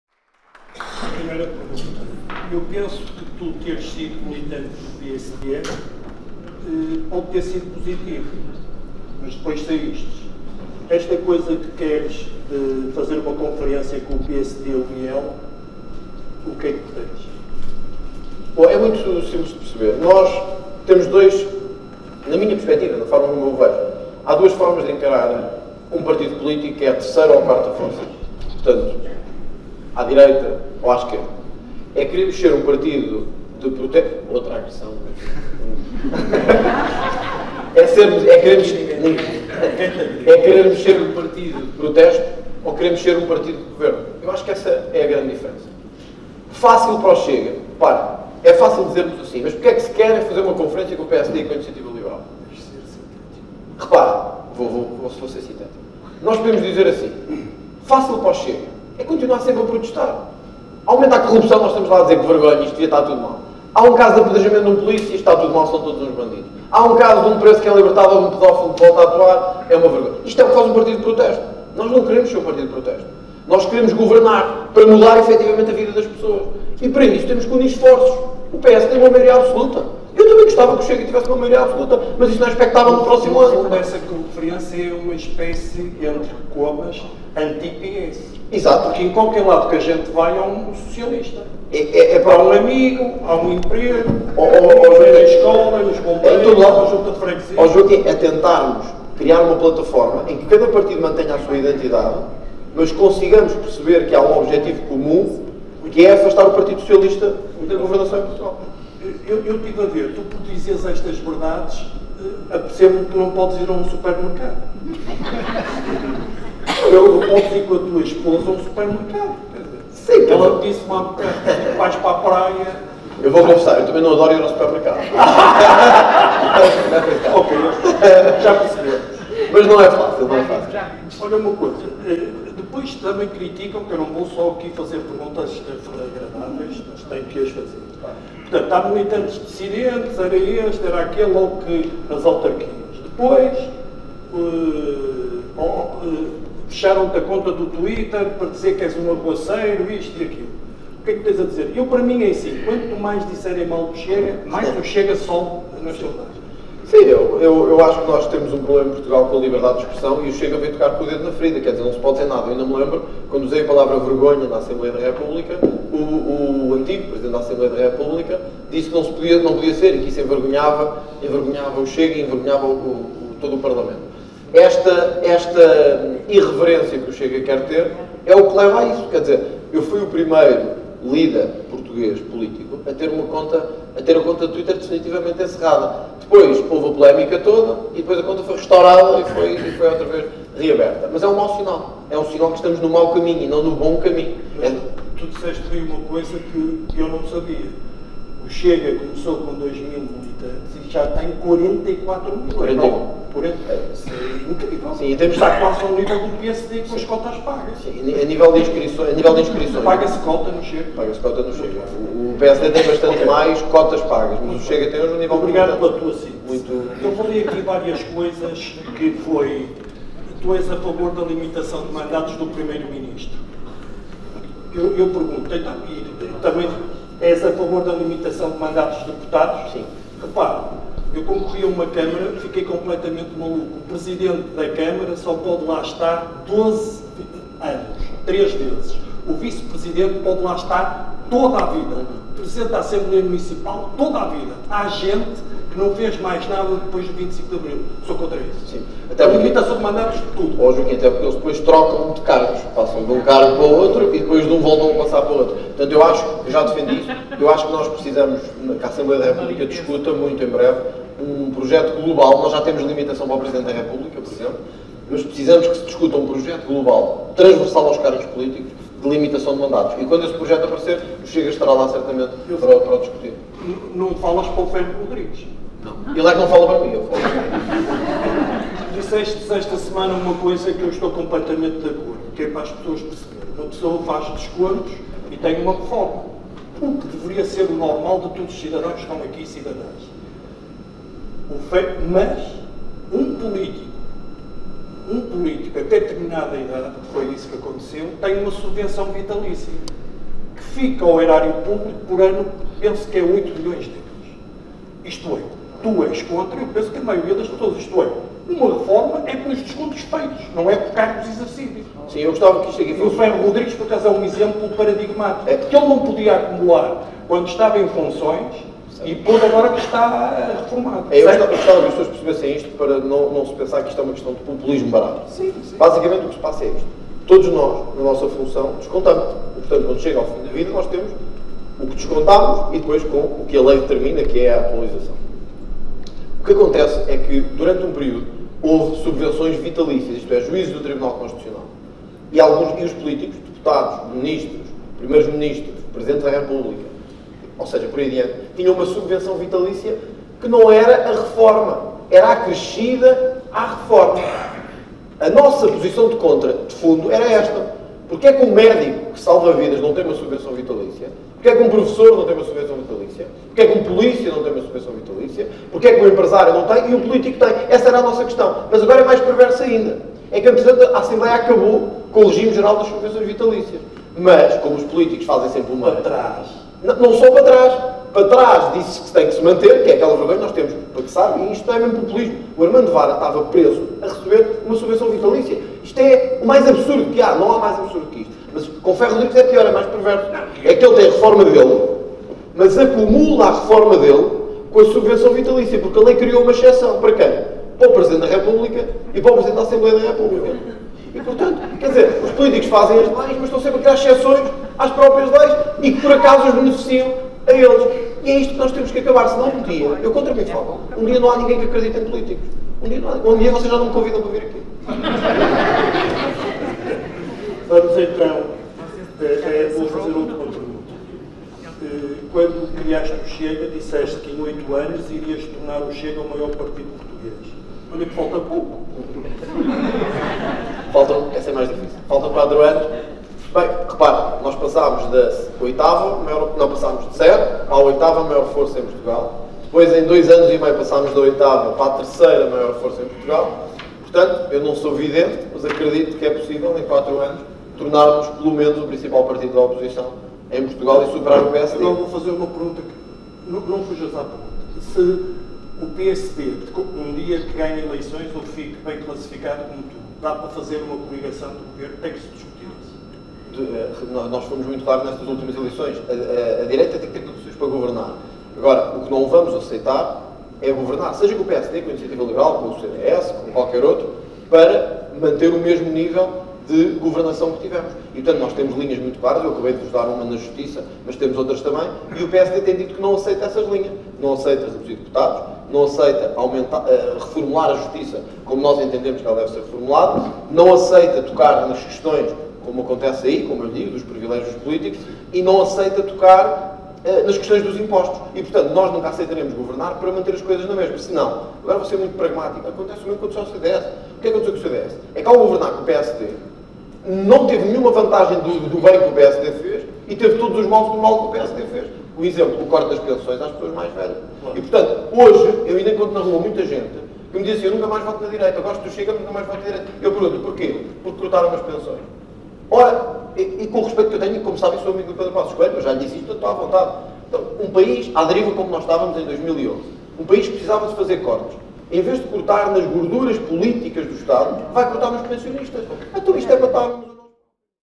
André Ventura e Joaquim Jorge em conversa
Debate completo no canal Grupo dos pensadores.